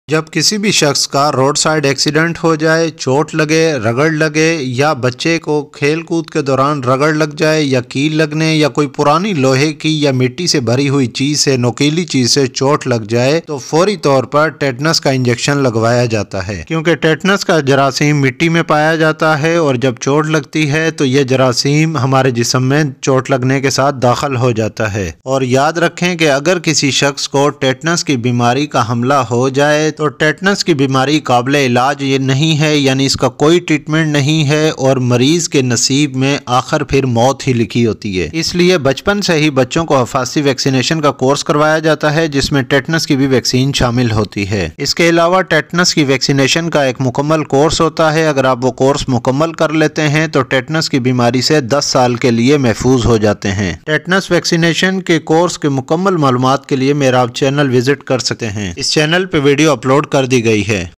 Imatate injection uses | tetnus injection uses | tetanus in pregnancy| benefits of tt injection
In this video I will explain about tetnus injection benefits dosage administration immunization schedule and protection from tetnus disease for 10 years.